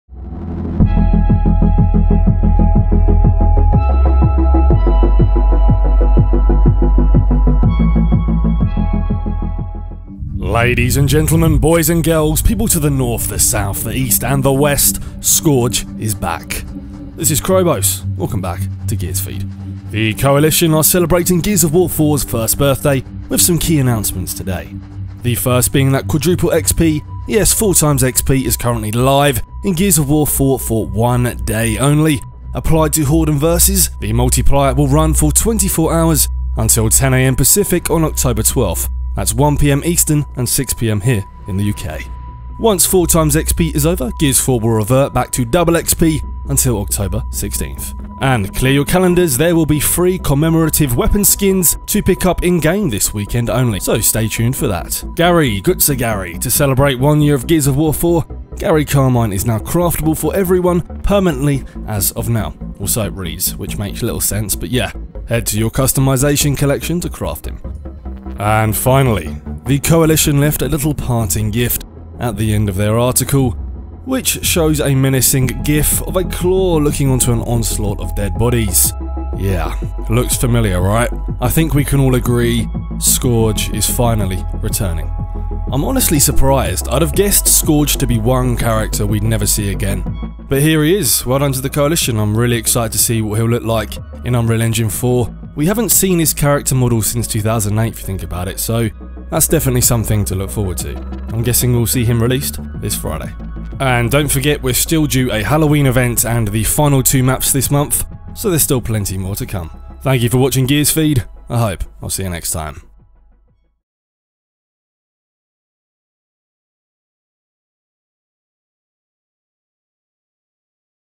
Gears of War 4 NEWS: SKORGE is back!
4XP,  SKORGE & more as TC celebrate one year of Gears of War 4